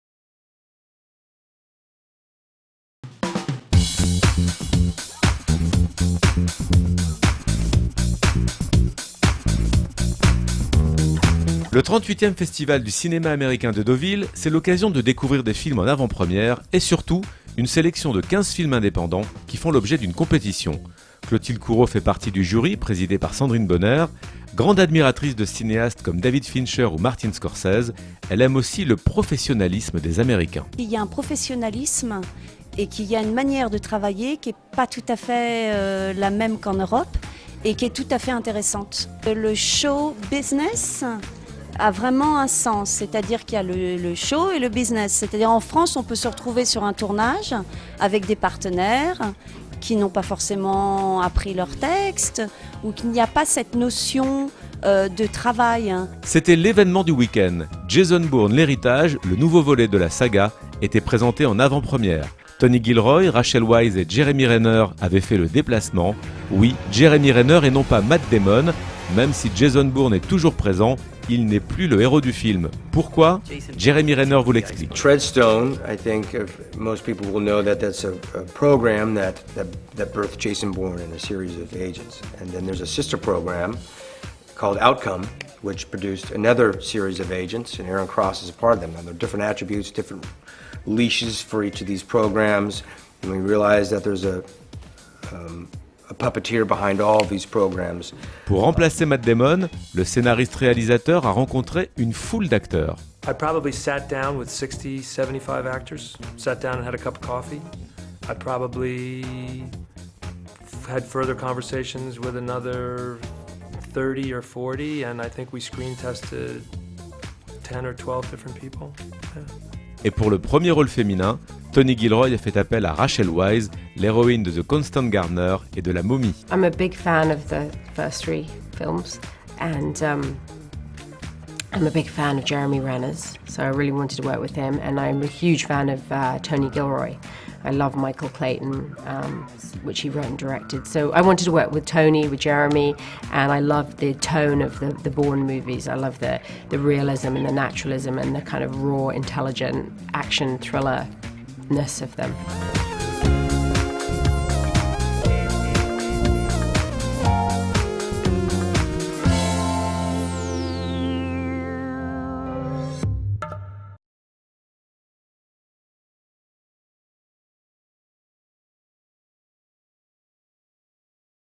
DEAUVILLE 2012: L'ouverture (Clotilde Courau - Bourne Legacy )
Jeremy Renner, Clotilde Courau, Tony Gilroy, Rachel Weisz ... ils étaient tous à notre micro pour ce premier week-end de Deauville !